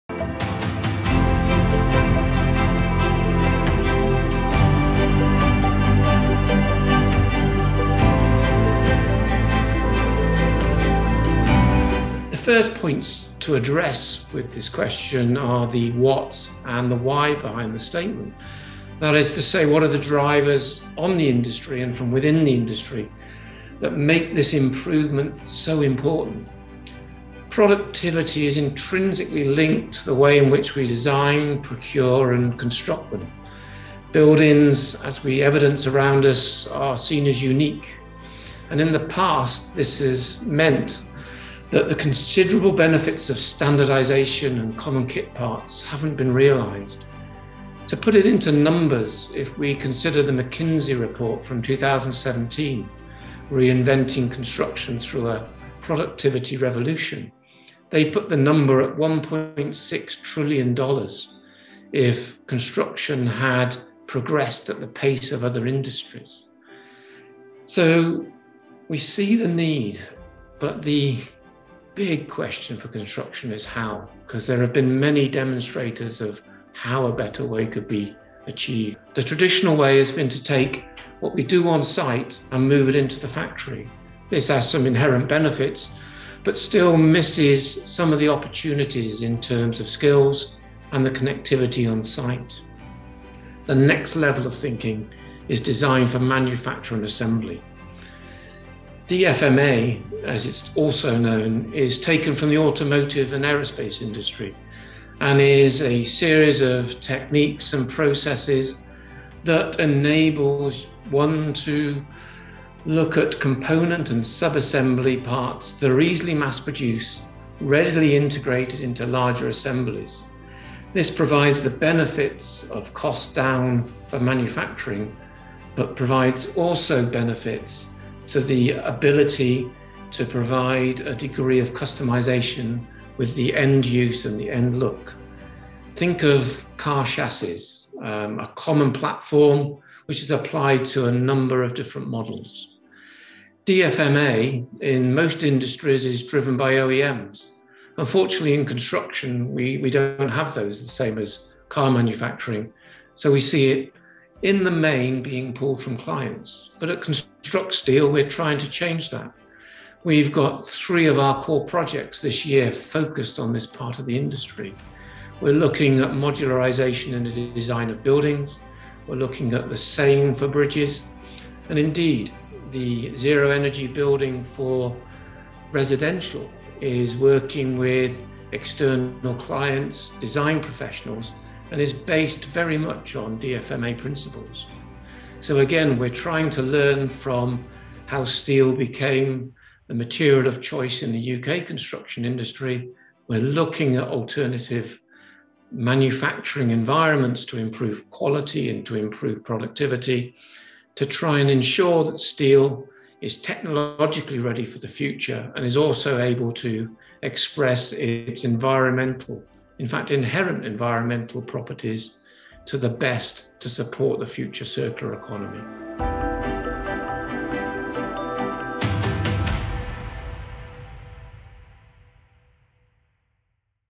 Jonathan Clemens – steel & productivity in the construction industry
Jonathan Clemens shares his thoughts on how steel can help increase productivity in the construction industry.